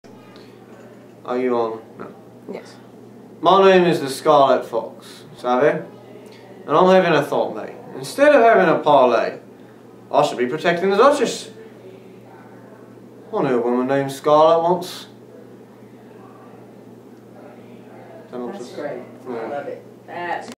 The Scarlet Fox -- Jack Sparrow (Boom, son.)
I was to say as Jack Sparrow would say, "My name is The Scarlet Fox. As the "Sword of the King" I will protect the duchess and vanquish evil doers." But Jack Sparrow's not all there at times...